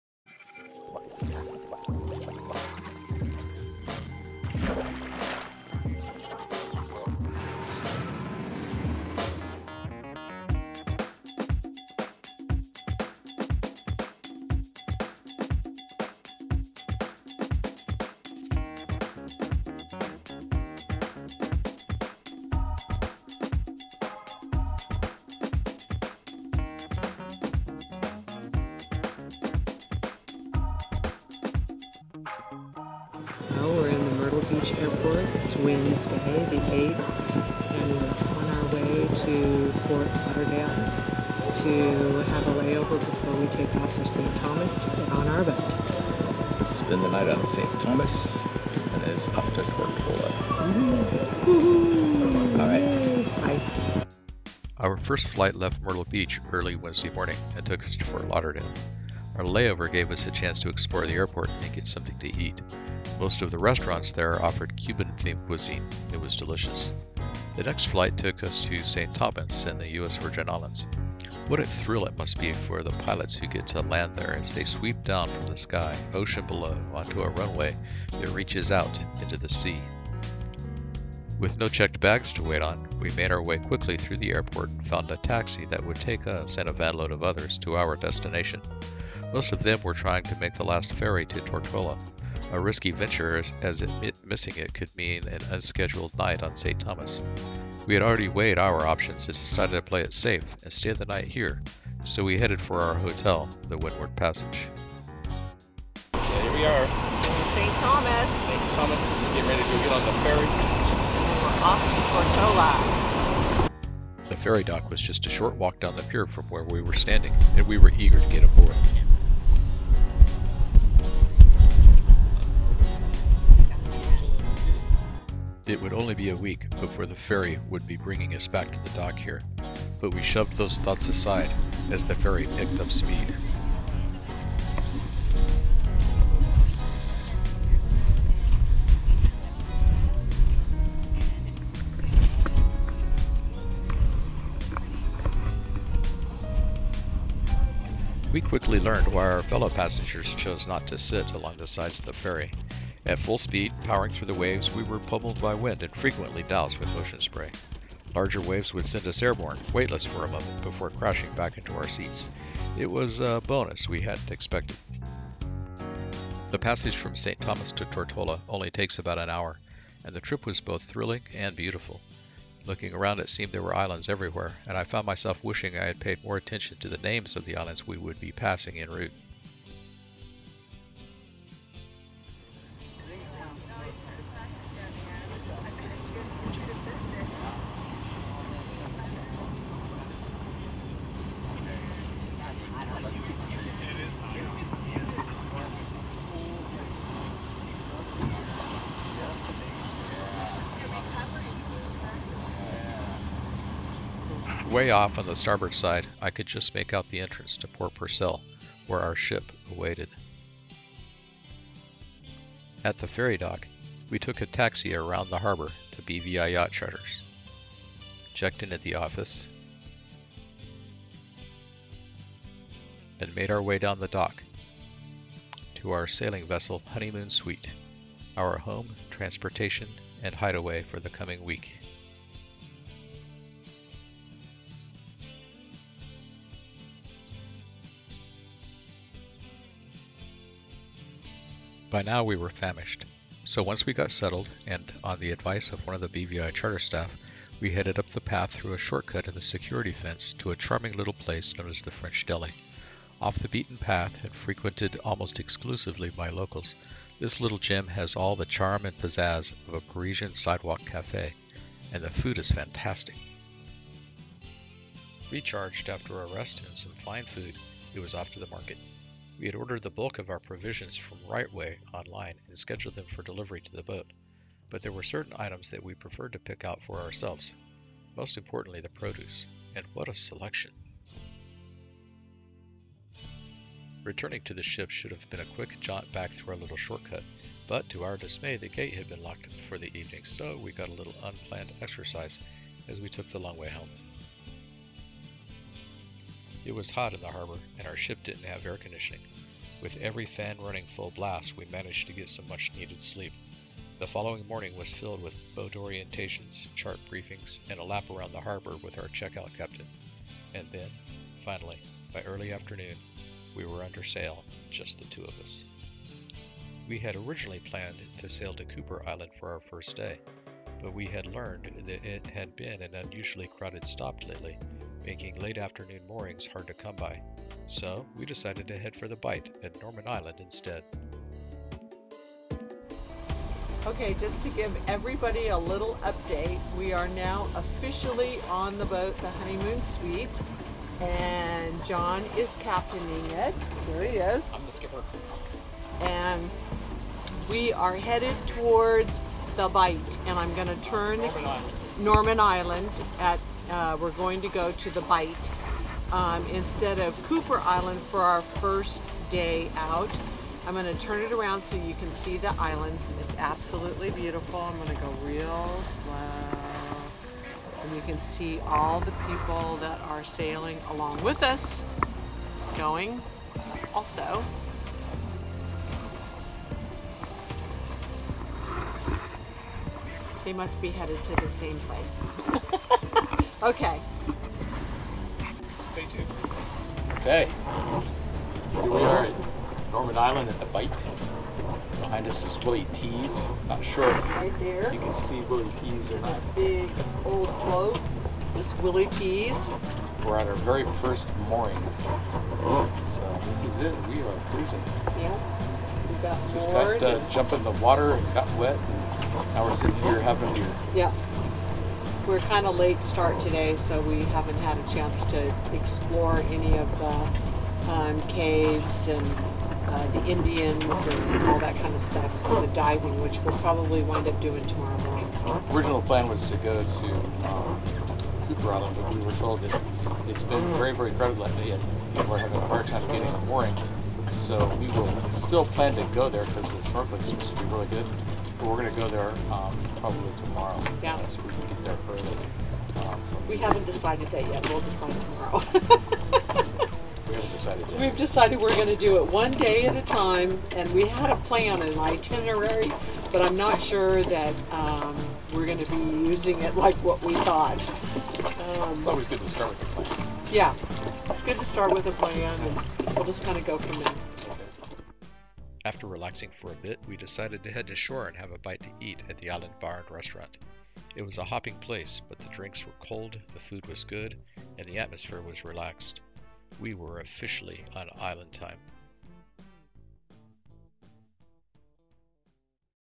02 John & Stephanie Sailing
Our very first bare-boat sailing adventure starts early on a Wednesday Morning in June 2016 at the Myrtle Beach Airport.  We fly to Ft Lauderdale and then on to St Thomas where we spend the night before taking the ferry to Tortola.  We grab some dinner at the French Deli, finalize our provisions and do a sleep aboard on our boat the night before our charter begins.  The following day we spend the morning with orientations and briefings before sailing to Norman Island - check it out . . .